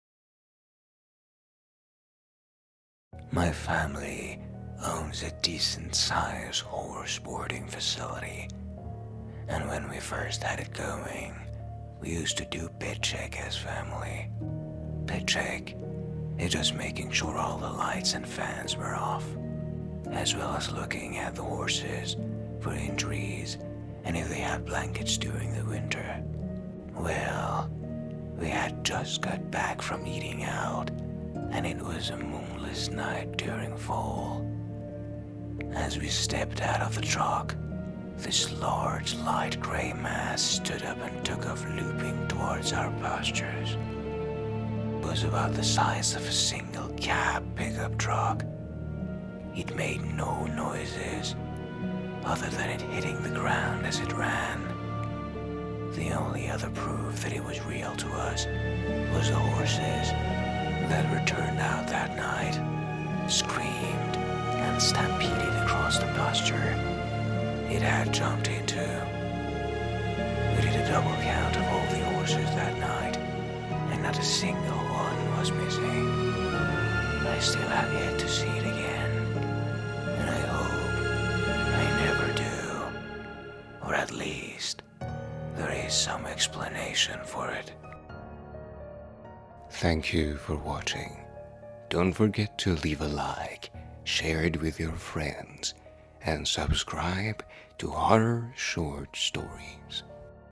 The Enigmatic Gray Mass: A Terrifying Encounter at the Horse Boarding Facility | Horror Story
Join us for a bone-chilling tale of a family's unsettling bed check at their horse boarding facility. On a moonless fall night, they encounter a mysterious, light gray mass of immense size that silently lopes through the pastures, sending the horses into a frenzy. With no explanation in sight, the family is left haunted by the enigmatic presence they witnessed that night. Will they ever uncover the truth behind this eerie encounter?

If you want to see more scary videos